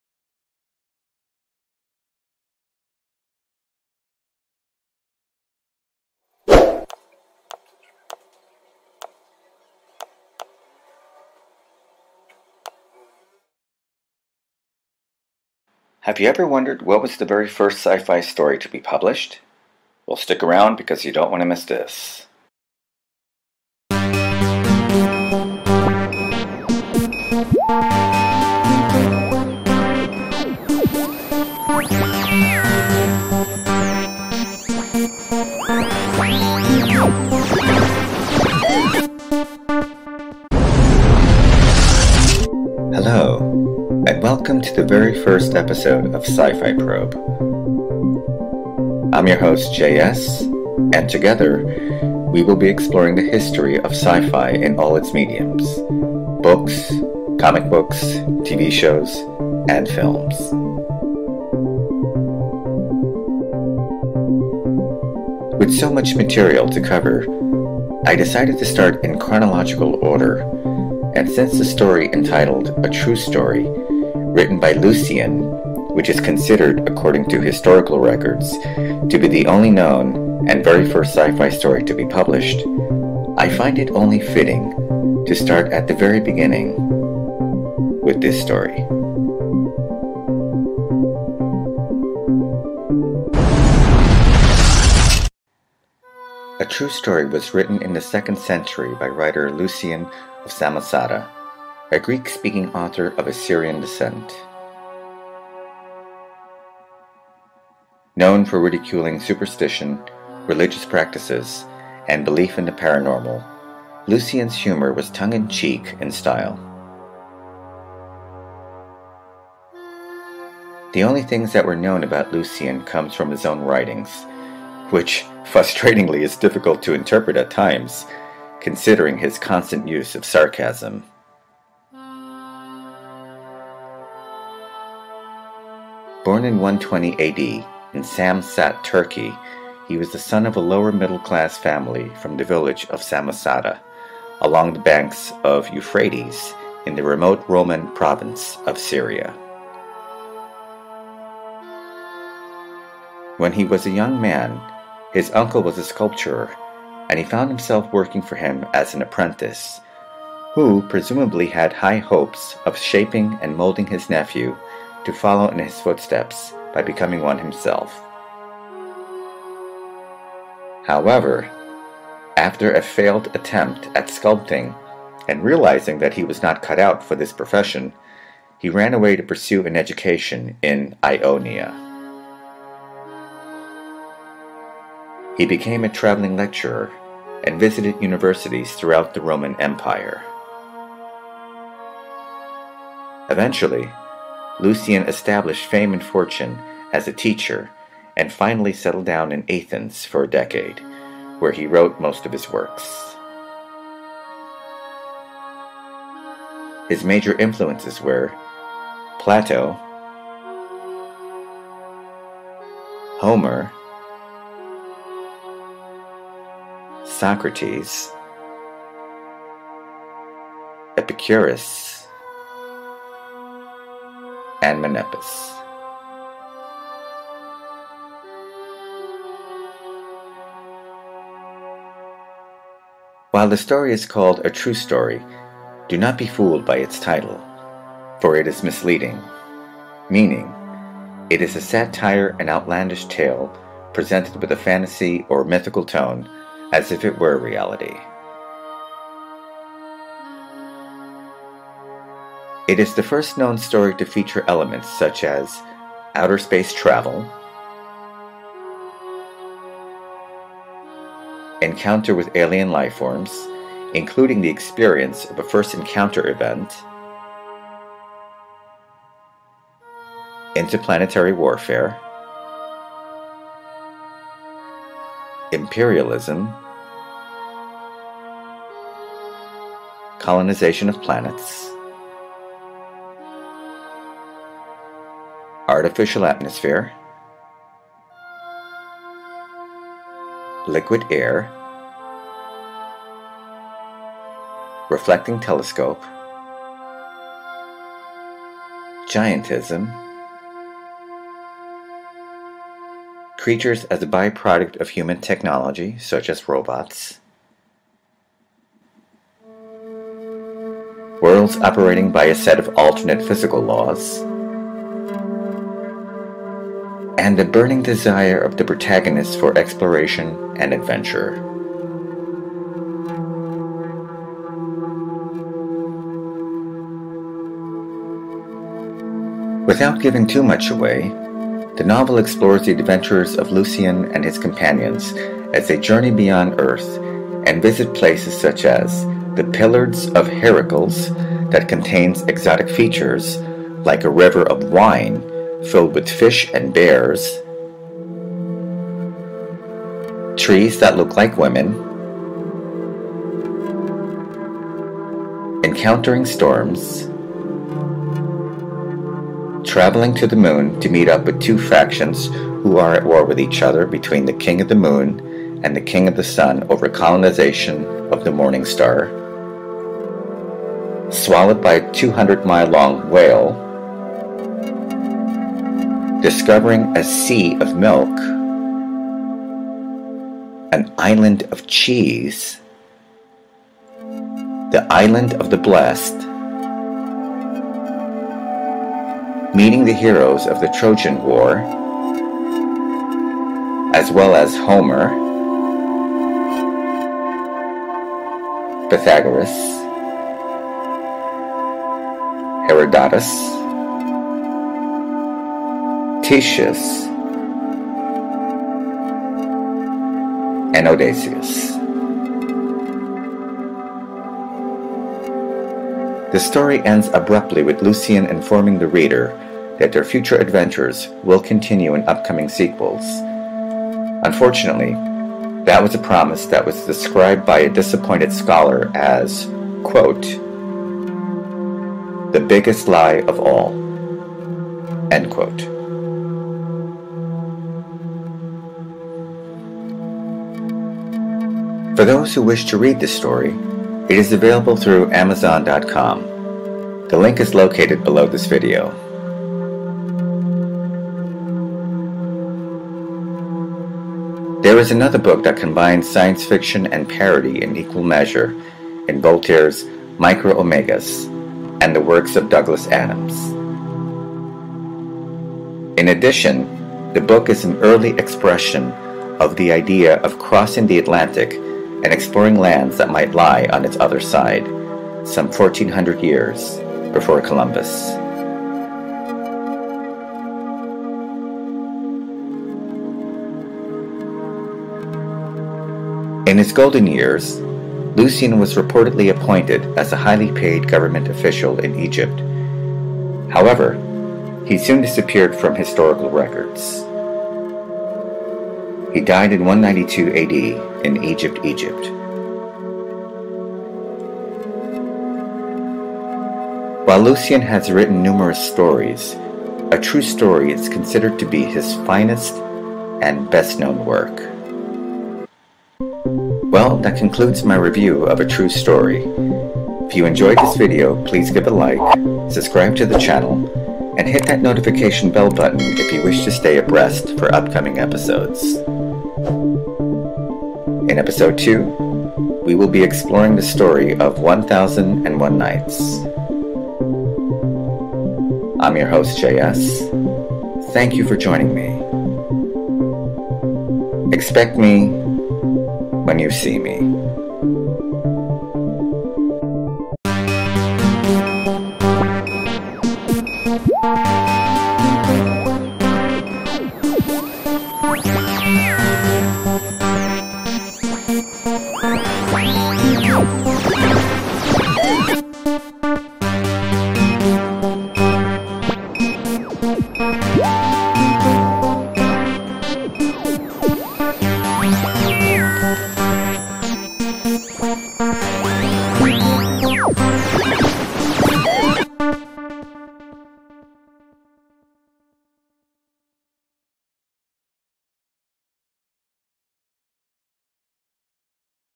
Sci-Fi Probe Episode # 1 - "A True Story" Review
"A True Story" is the first official sci-fi story to be written and published by Greek author Lucian in the second century A.D. The story is a satire consisting of outlandish tales featuring outer space travel, encountering aliens, trees that look like women, and a bunch of other outrageous beings in which they are presented as fantastical or mythical occurrences as if they were reality.